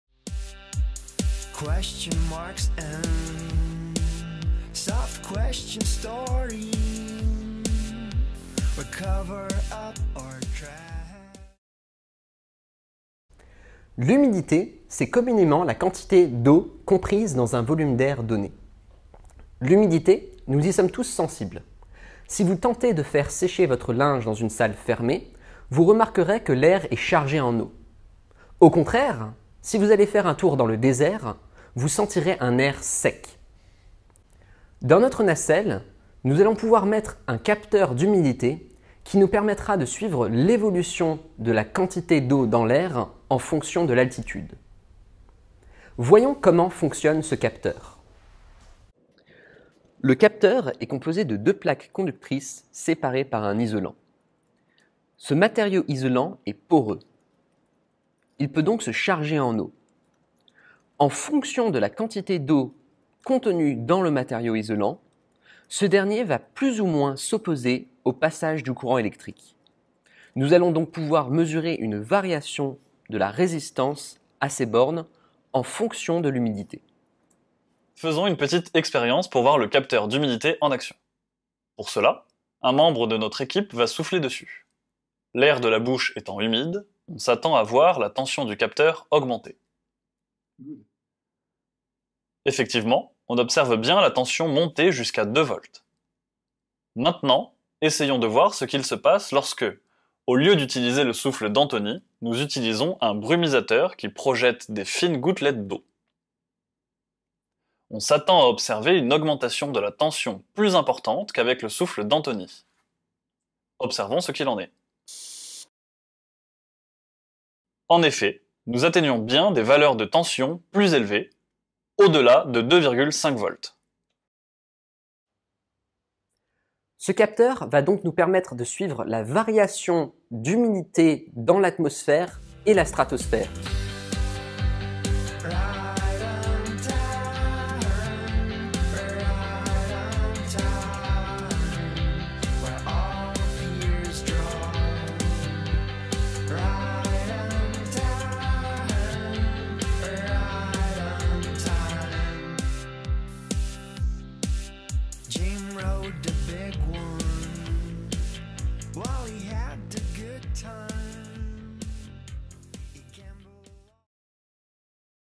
Le capteur d'humidité
Présentation du capteur d'humidité, présent dans notre nacelle de ballon stratosphérique.